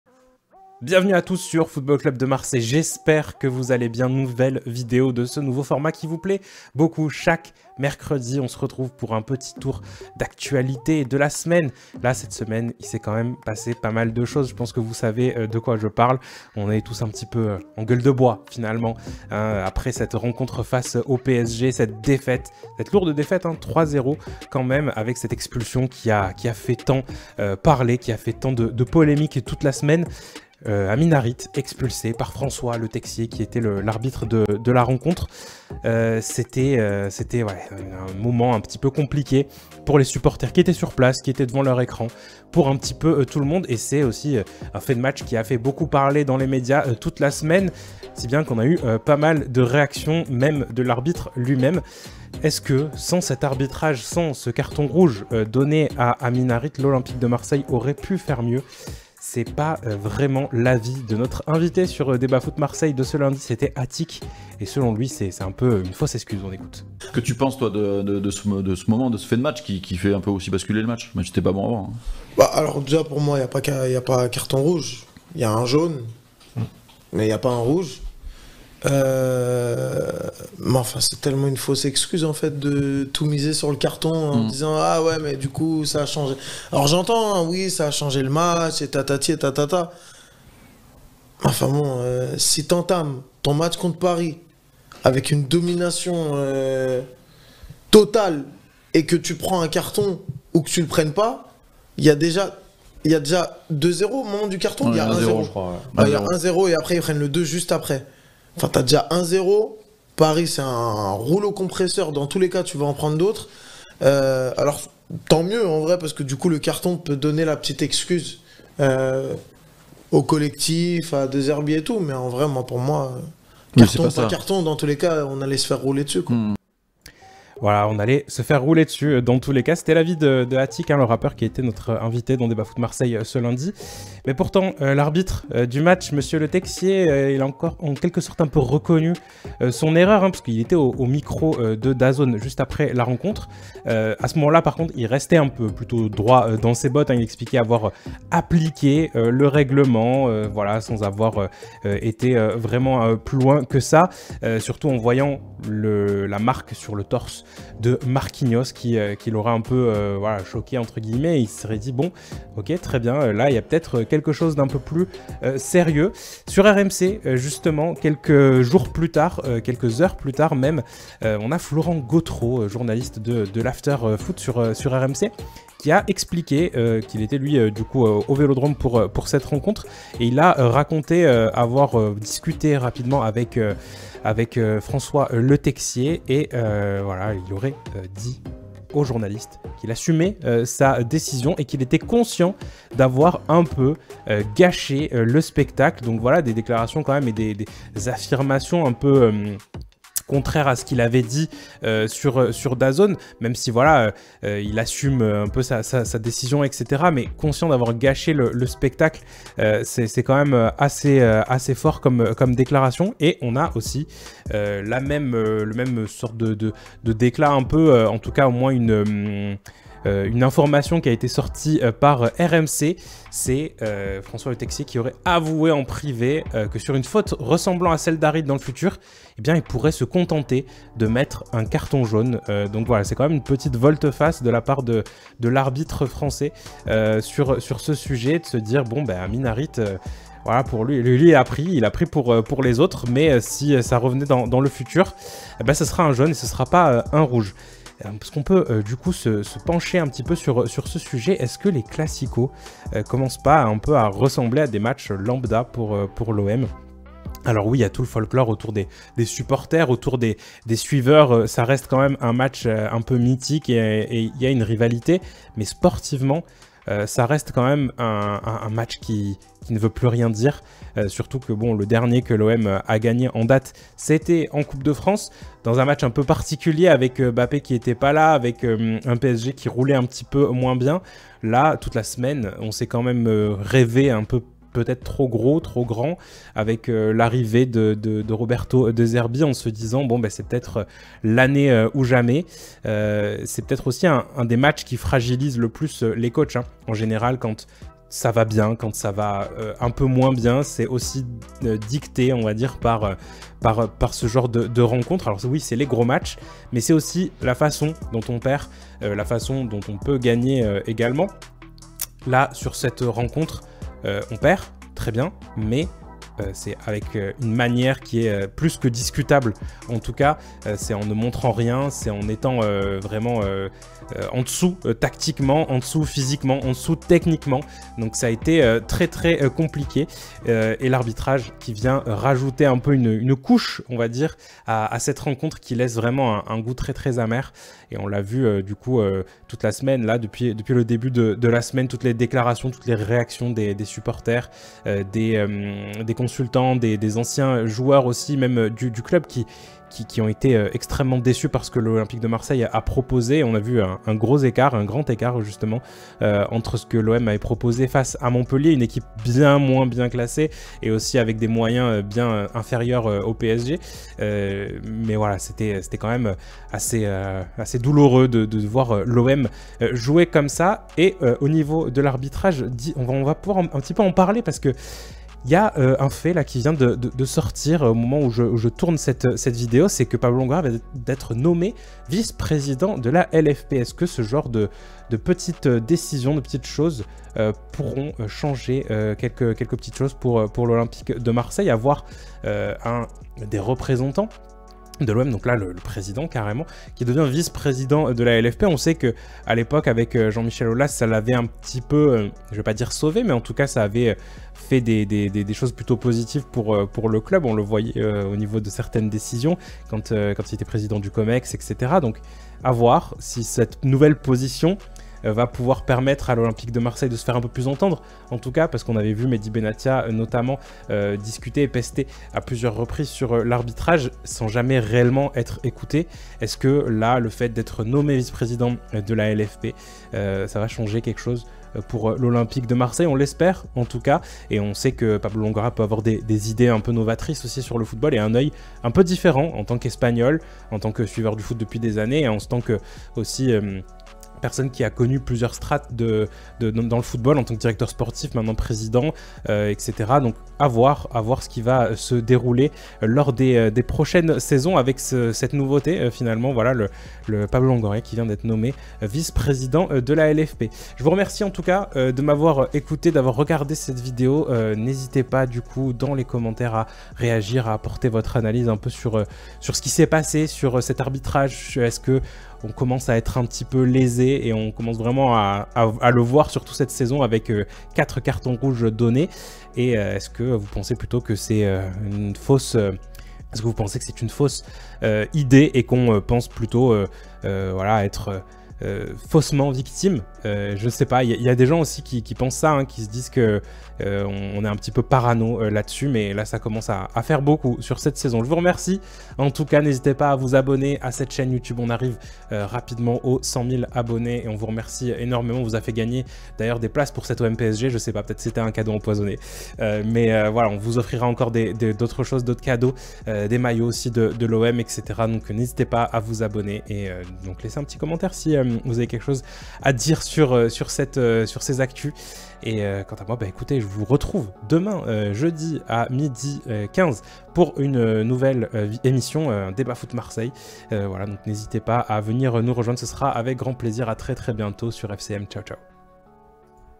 OM / PSG : LE ROUGE POLEMIQUE DE LETEXIER ! Longoria VICE-PRESIDENT DE LA LFP?
Toute l'Actu de l'OM SANS PUBLICTE avec du contenu EXCLUSIF c'est sur FCM